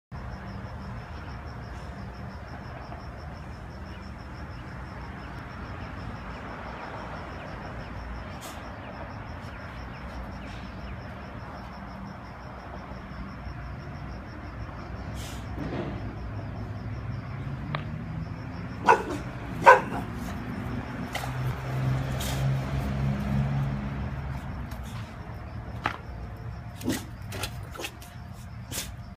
Boxers Trying to get onto a Hammock
Fred and Lloyd being silly, as usual.